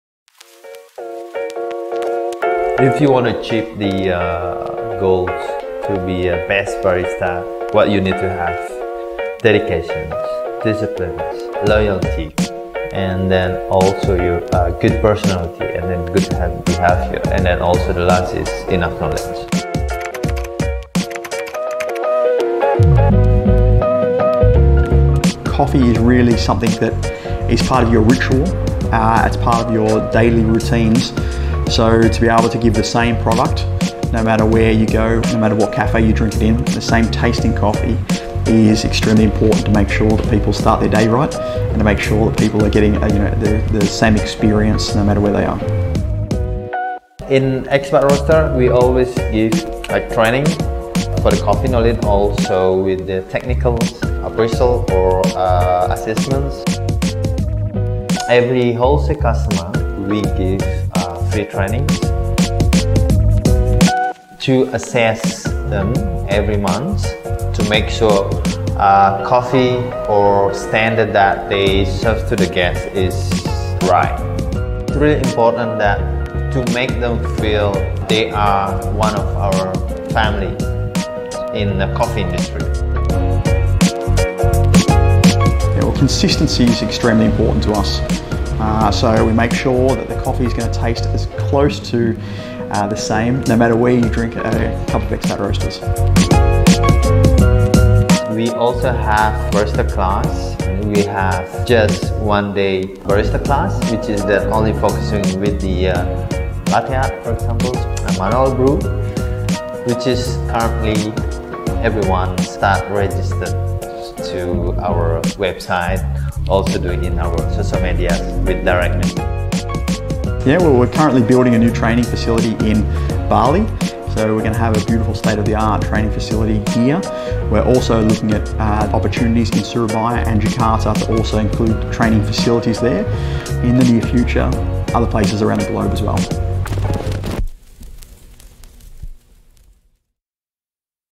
Aiming Consistency Through Barista Training - Expat. Roasters
As a Barista, producing a consistent cup every day is an essential part of the job. Through Barista training every barista knows the coffee's parameters and keeps up with the required standards.

At Expat. Roasters, we help our wholesale customers by providing barista training both at their venue or at our training center. The same goes for our in-house baristas as well.

Keen to unlock your barista skill?

Where to buy our specialty coffee